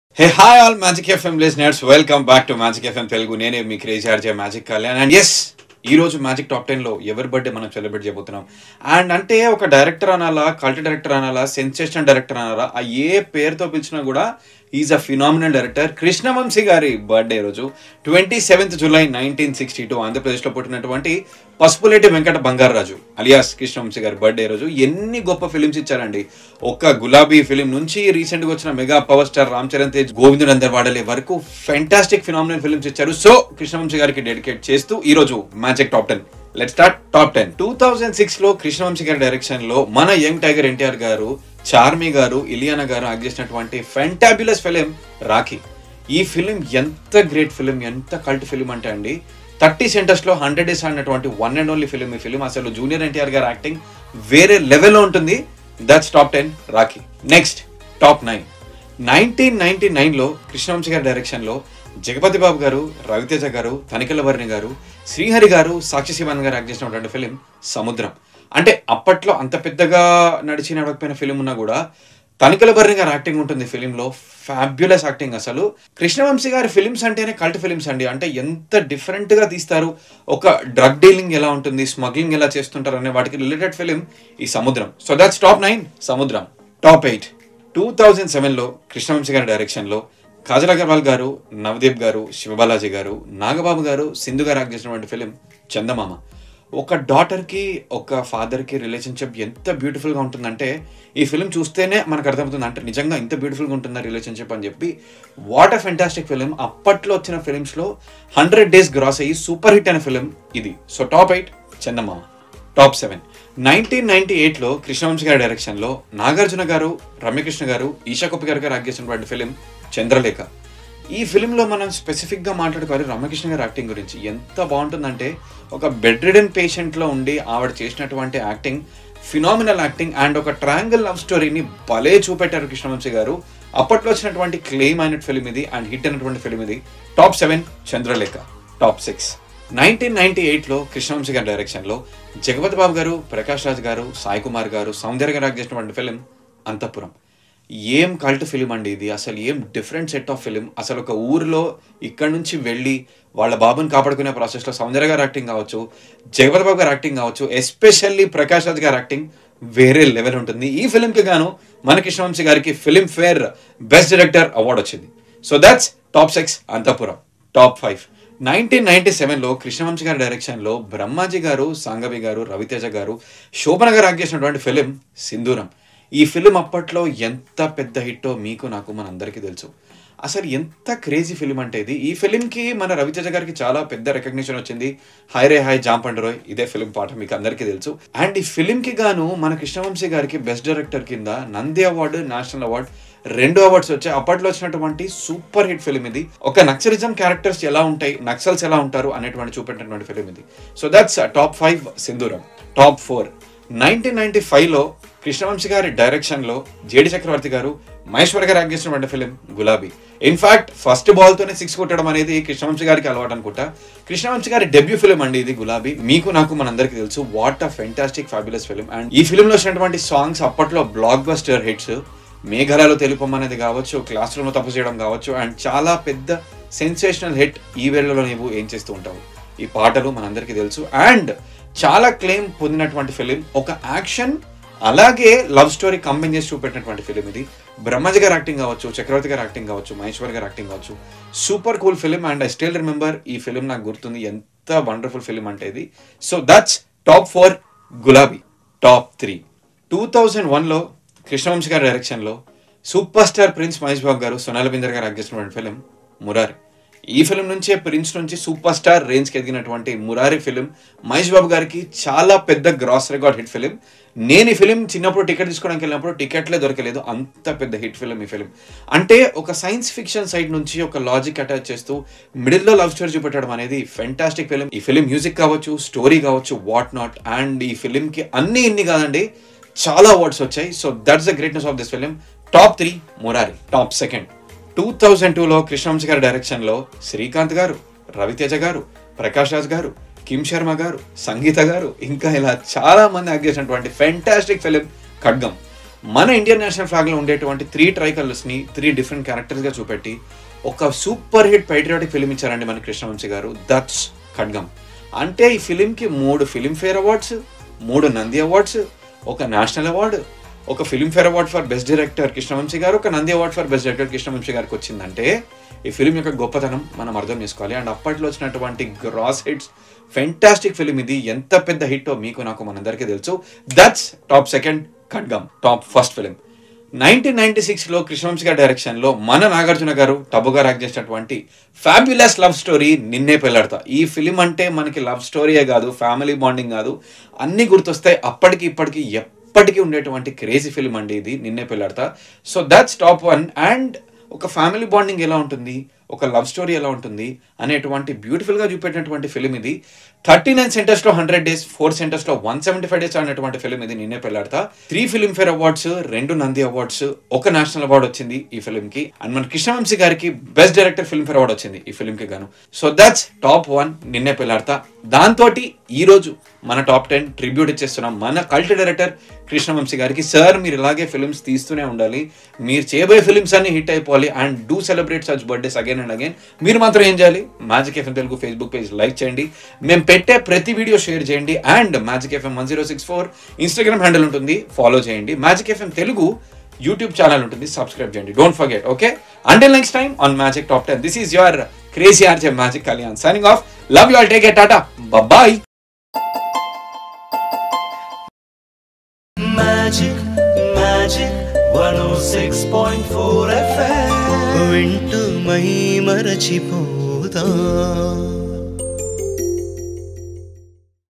Top 10 Krishna Vamsi Movies | RJ Kalyan | Magic FM Telugu
On the occasion of the creative director, Krishna Vamsi Garu's birthday, RJ Kalyan presents to you the top 10 movies of Krishna Vamsi.